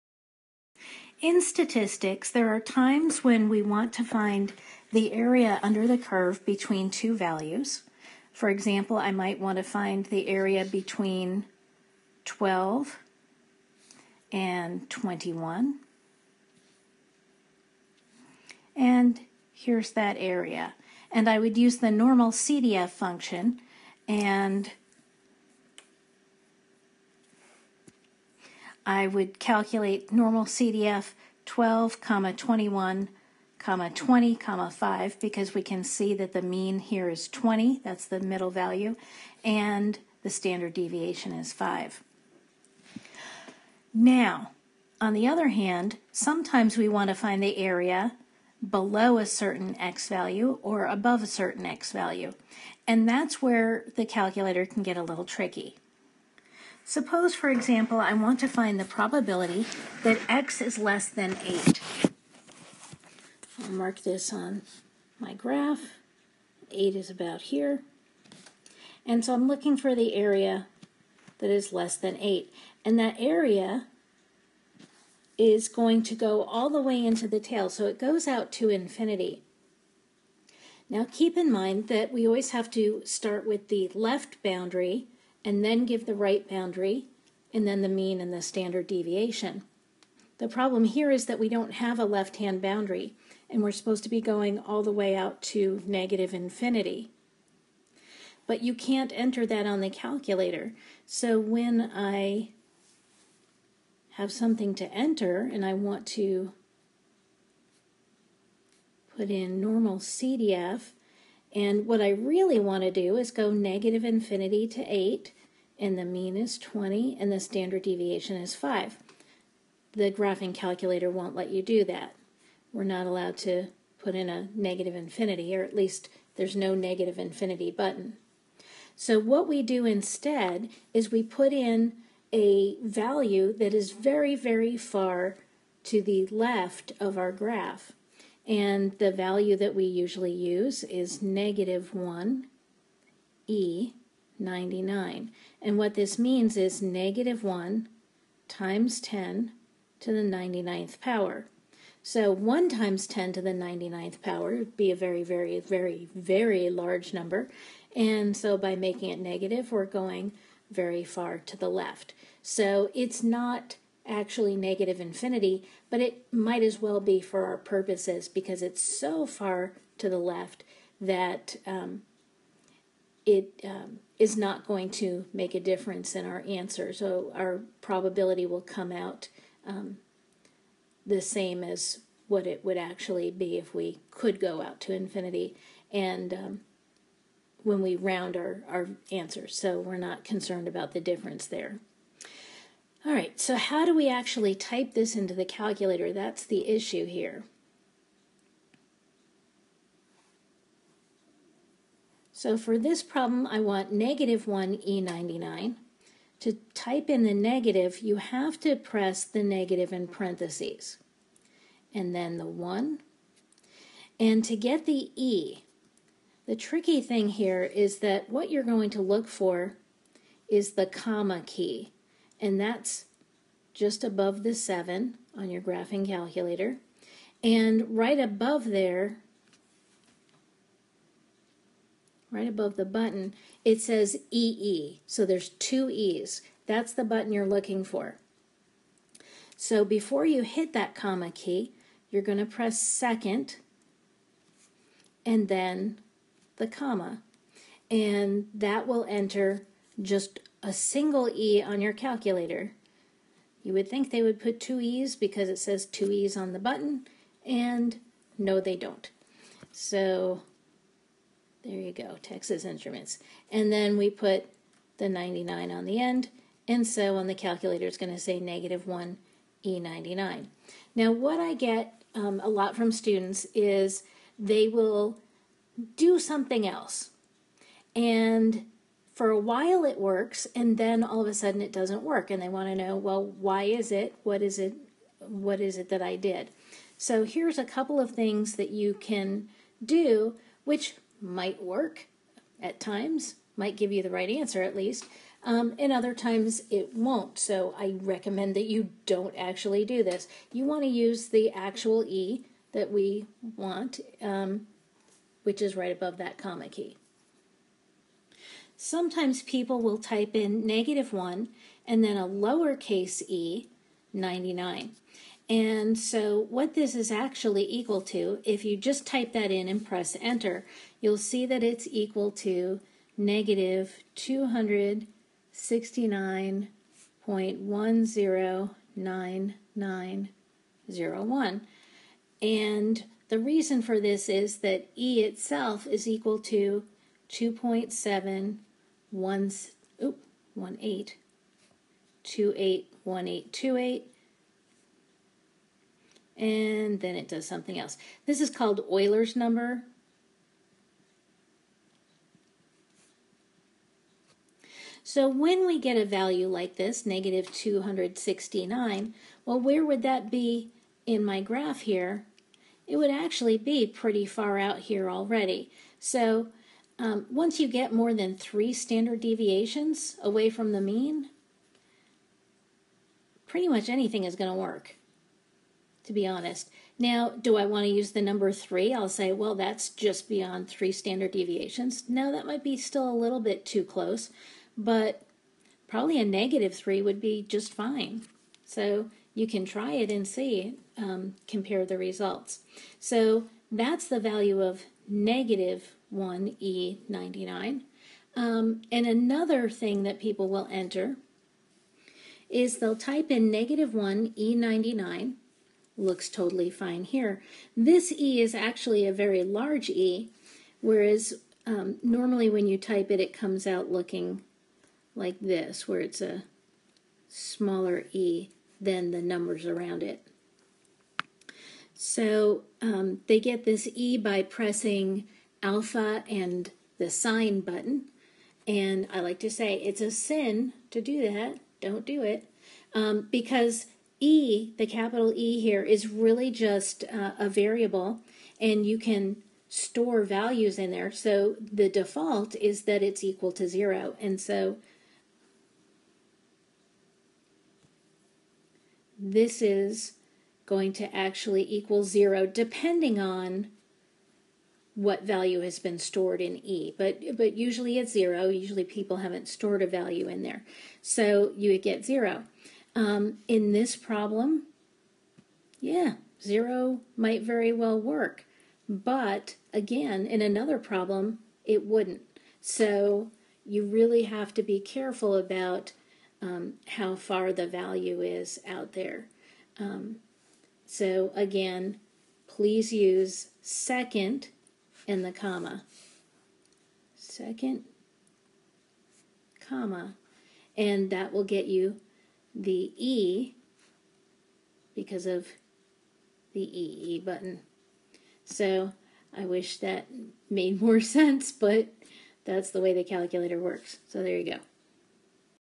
Section 6-1: Statistics - -1E99 and 1E99
How to find a probability using the normal distribution when there is only one boundary value.